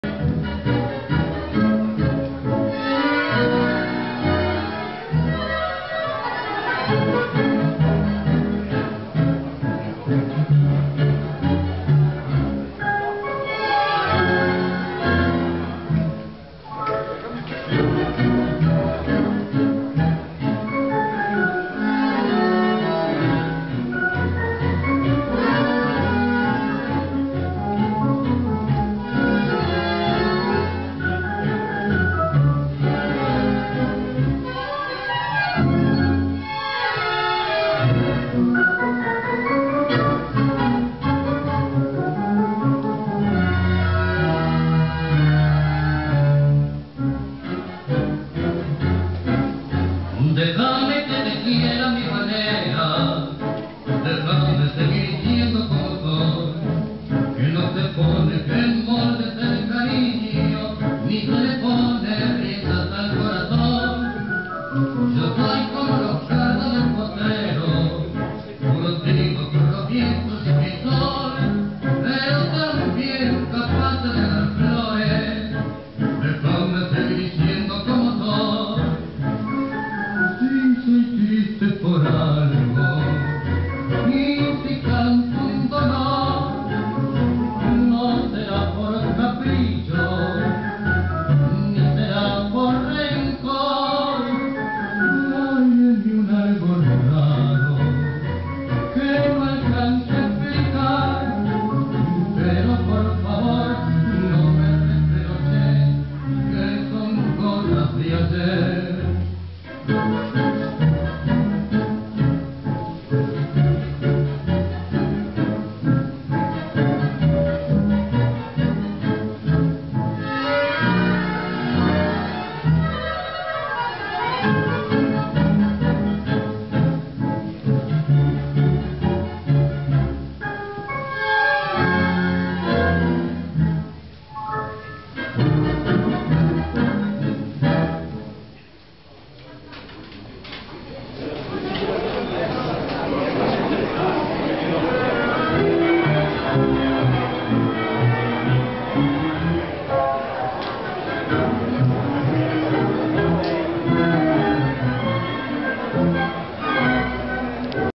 LES CIGALES 7 (Sainte Colombe - FR) 14-17 May 2015
Il primo Tango della tanda ... la pista si riempie.
La première Tango de la tanda ... en remplissant la piste.
The first Tango of the tanda ... filling the track.